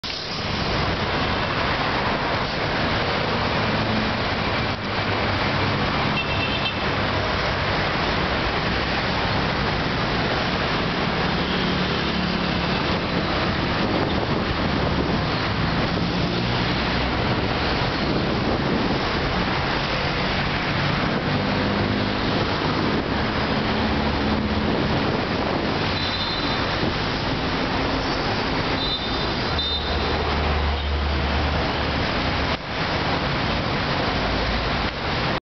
Av. Pres. Vargas, Rio de Janeiro
Av. Presidente Vargas, al fondo la Iglesia de la Candelaria. Rio de Janeiro, Brasil. 17 de diciembre del 2012, 1.48pm.